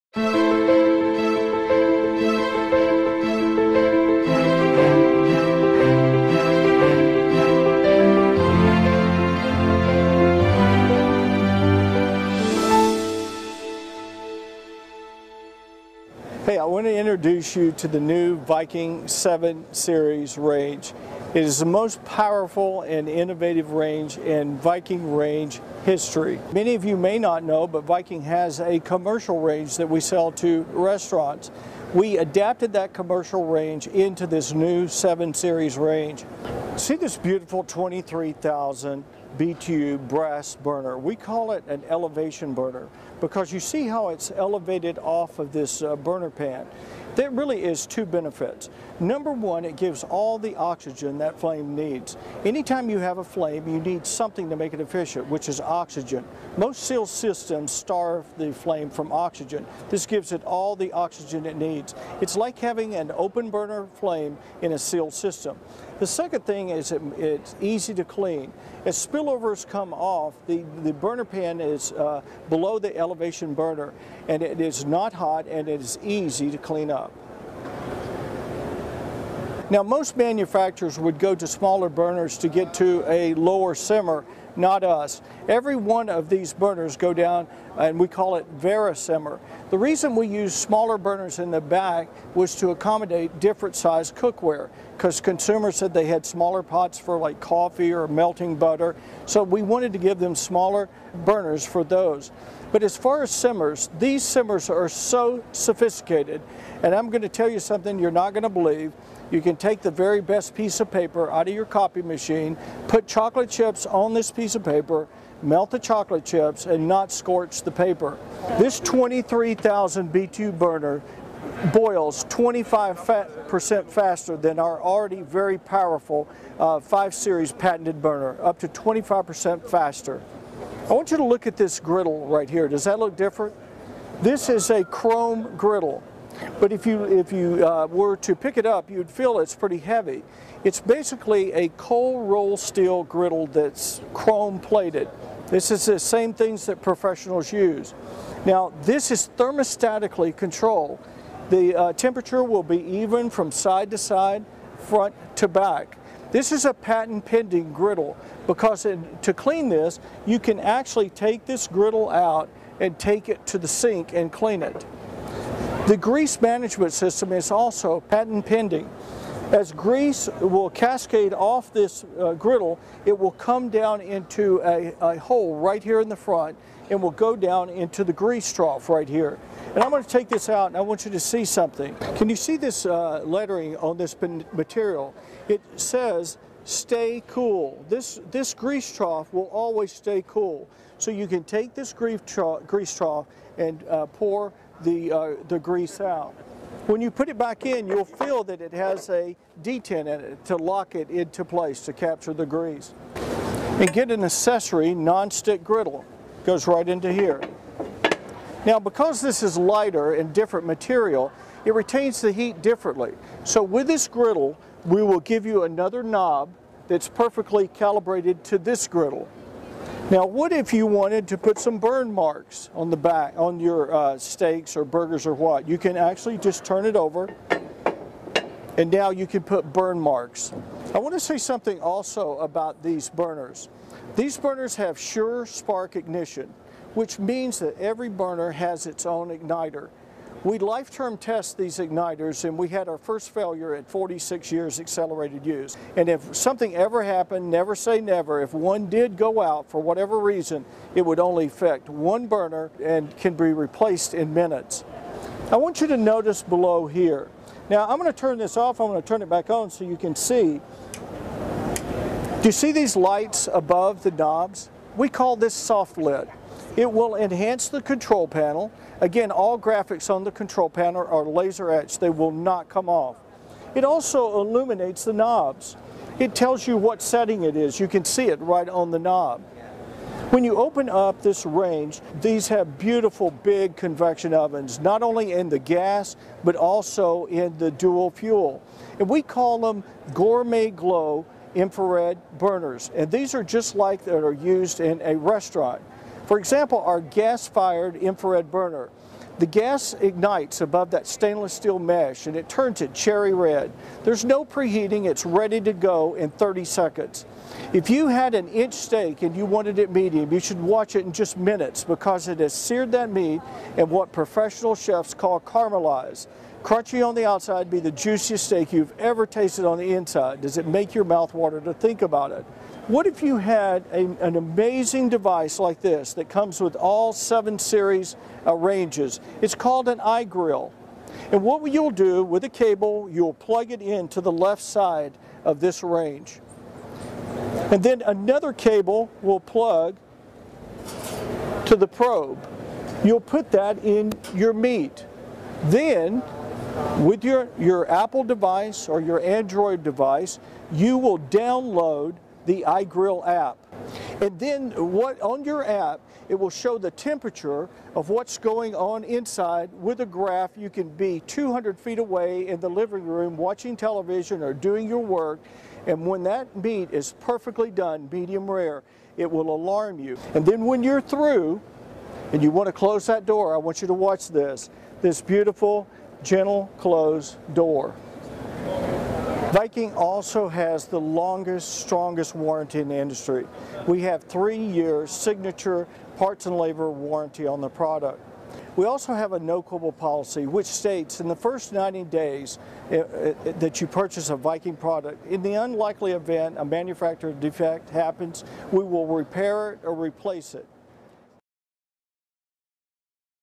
Viking Professional 7 Series Dual Fuel Freestanding Range
The Professional 7 Series 48" VDR7486GSSLP Dual Fuel, Liquid Propane Range features 6 sealed burners and a griddle. The VariSimmer Setting on ALL burners provides gentle, even simmering at precisely low cooking temperatures, and the SureSpark ignition system ensures automatic ignition and re-ignition in case the burners extinguish while cooking. The SoftLit LED Lights illuminate knobs and accent the control panel, adding to the professional look of the range.